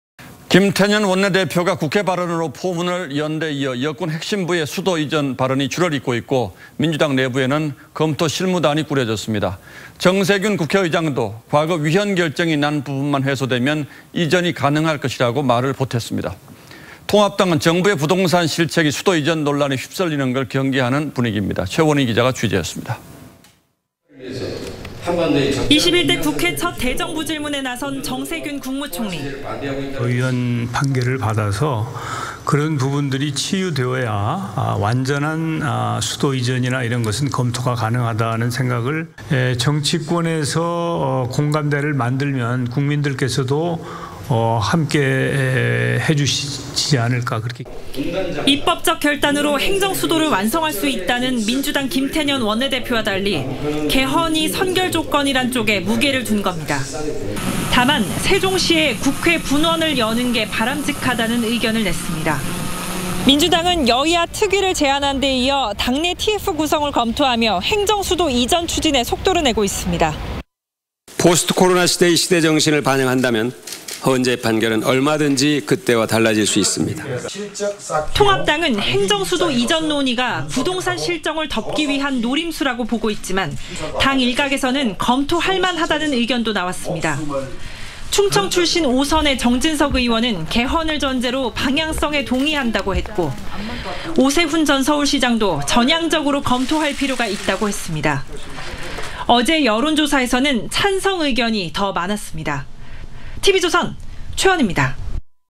정세균 총리 "행정수도 위헌판결 먼저 치유돼야"…여당 "이전 추진단 설치"
김태년 원내대표가 국회 발언으로 포문을 연데 이어 여권 핵심부의 수도 이전 발언이 줄을 잇고 있고, 민주당 내부에 검토 실무단이 꾸려졌습니다. 정세균 국회의장도 과거 위헌 결정이 난 부분만 해소되면 이전이 가능할 것이라고 말을 보탰습니다. 통합당은 정부의 부동산 실책이 수도 이전 논란에 휩쓸리는 걸 경계하는 분위기입니다.